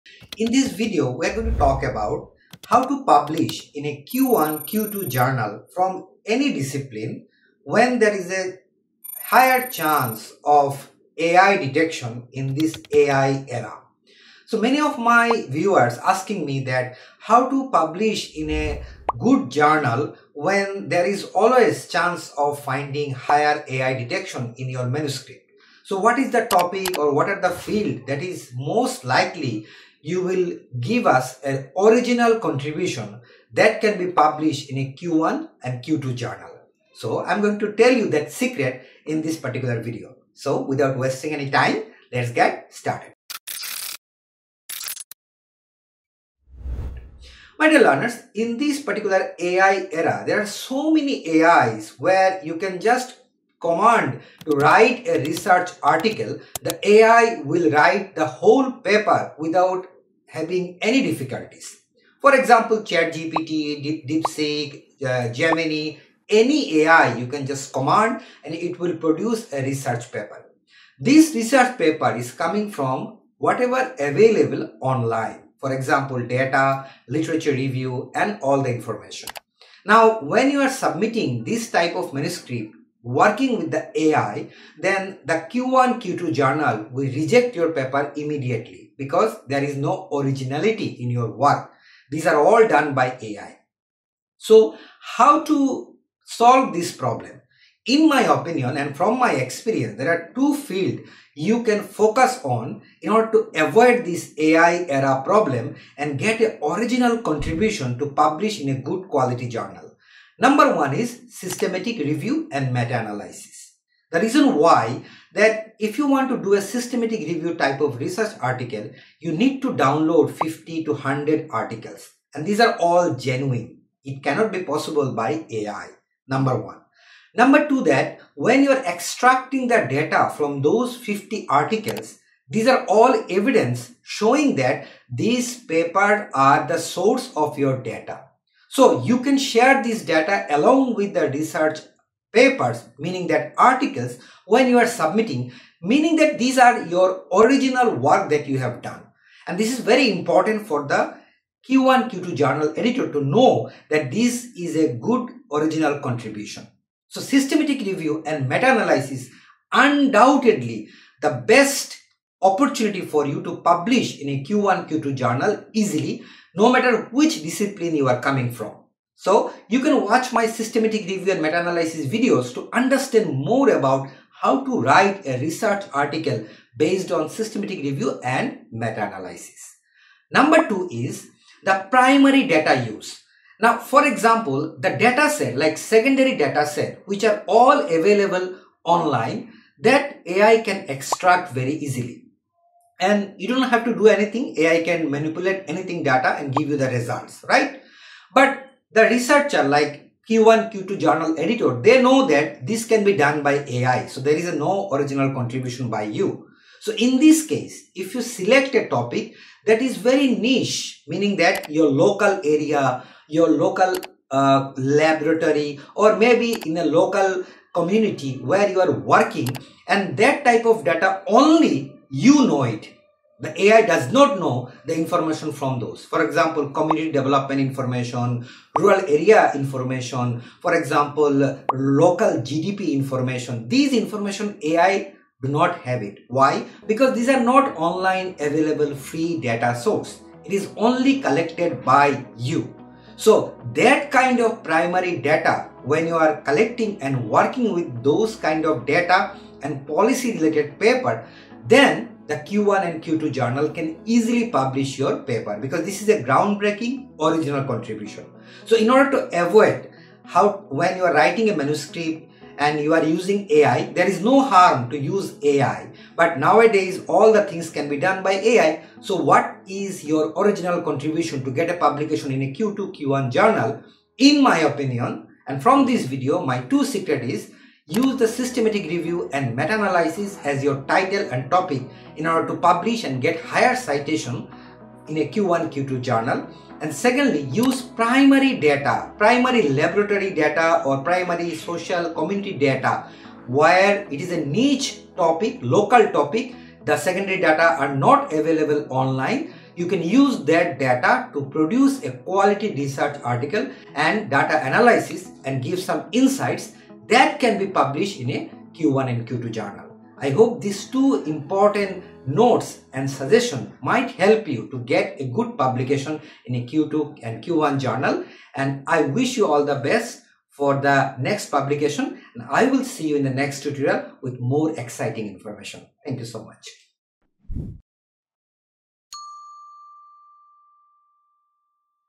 2 Secrets Nobody Tells You About Publishing in Q1 & Q2 Scopus Journals| Scopus Success Formula!
In this video, I reveal two guaranteed secrets that will help you create original contributions that AI cannot replicate:

Secret 1: Systematic Literature Review (SLR) & Meta-Analysis – A scientific way to synthesize existing research and produce high-value insights that journals love.

Secret 2: Primary Data from Niche Sub-Sectors – This involves collecting data from local communities, indigenous groups, local labs, and unique sectors that are not recognized by AI. This approach makes your research stand out as truly original and AI-proof.

Whether you are a PhD student, early-career researcher, or academic professional, these methods will boost your chances of acceptance in top-ranked Q1 & Q2 Scopus journals.

Watch until the end to learn how to combine these strategies and publish successfully in the AI age!